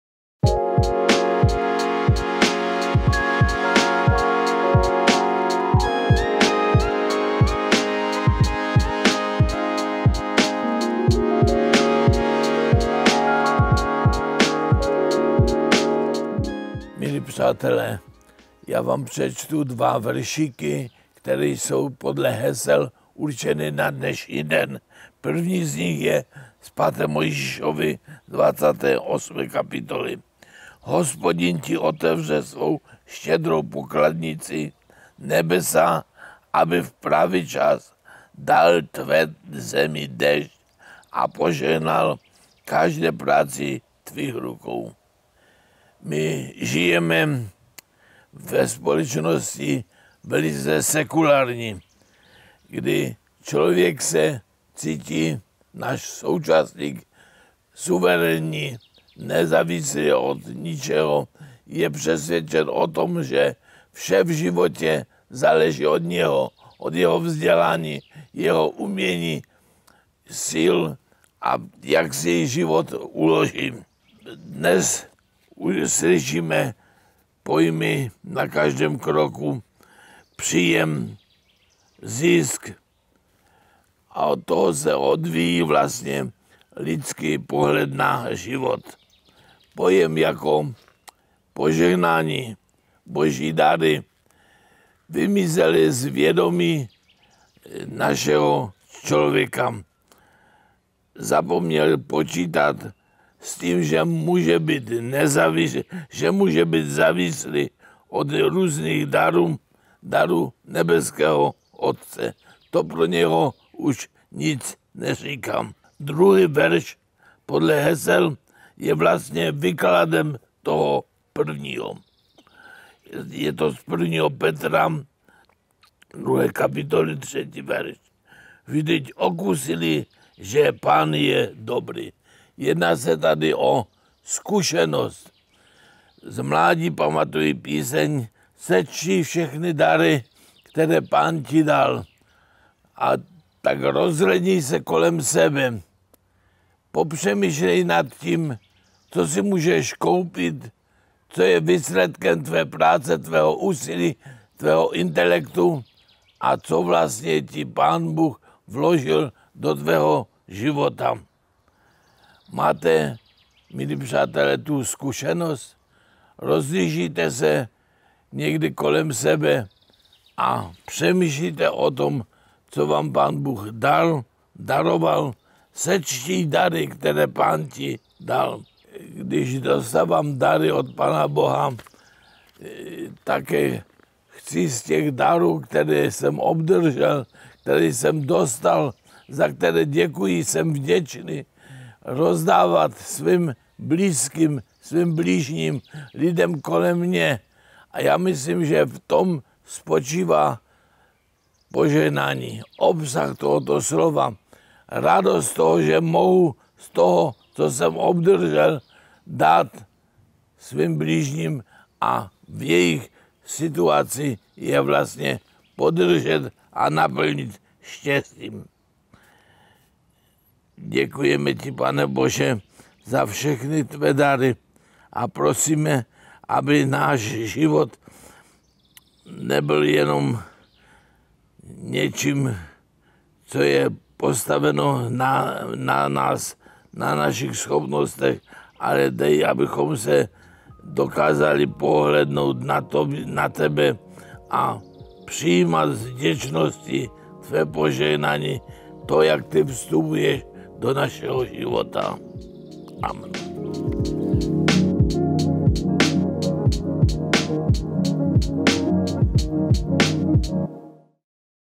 Slova naděje 17. 6. - Vladislav Volný, bp
Víte, co je to opravdové požehnání?
Pusťte si dnešní Slova naděje s emeritním biskupem Vladislavem Volným :)